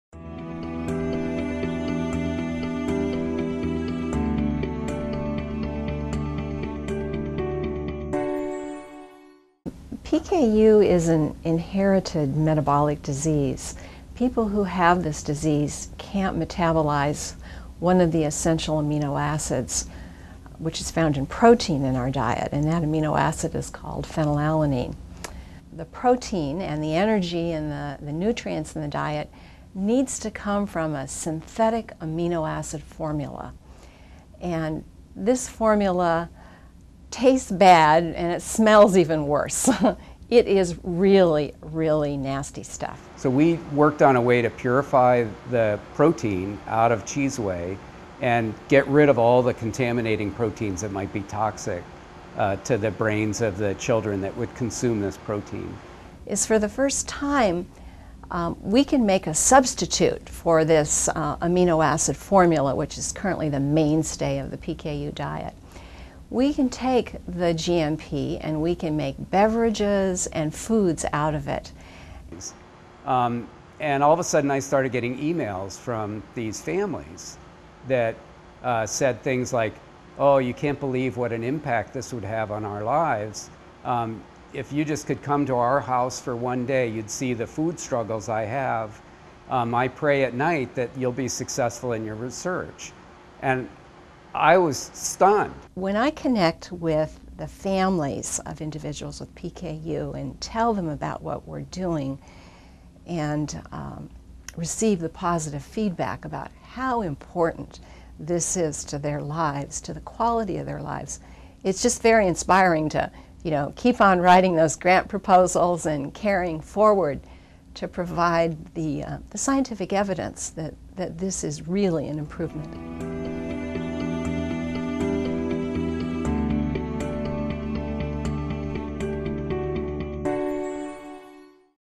The Genetic Disease PKU and a Medical Food Response
People afflicted with PKU can't digest a certain protein and end up with profound brain damage. Research at the University of Wisconsin College of Agricultural and Life Sciences responds.